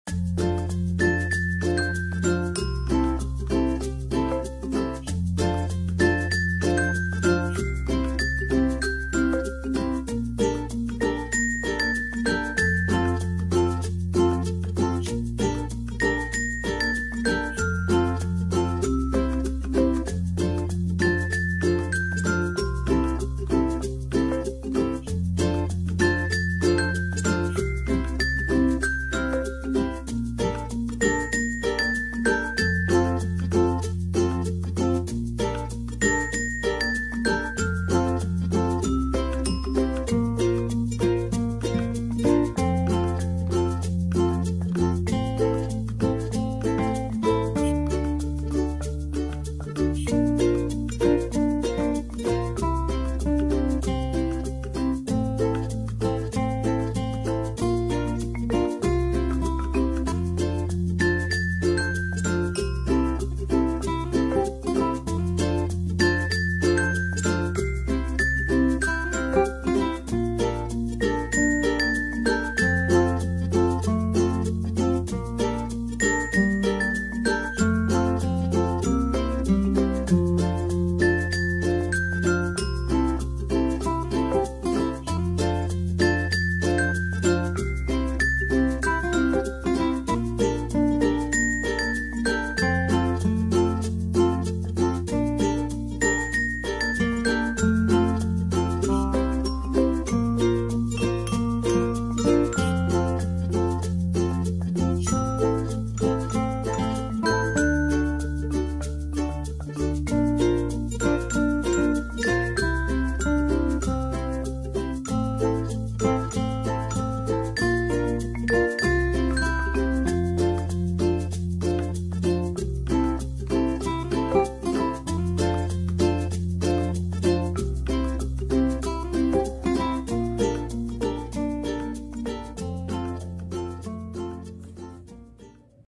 IDE BISNIS MINUMAN DARI YAKULT KEKINIAN
Bahan-Bahan:
1 buah mangga harummanis
1 saset nutisari mangga
1 botol yakult
150 ml susu full cream